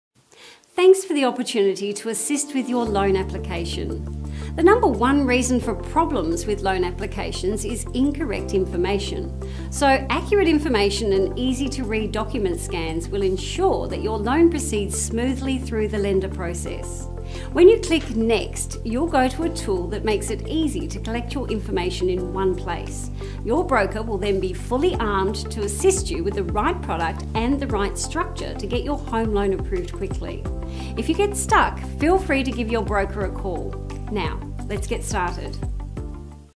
Tick Finance intro
Introduction on how to use this portal we have created to make things easier for you to give us your information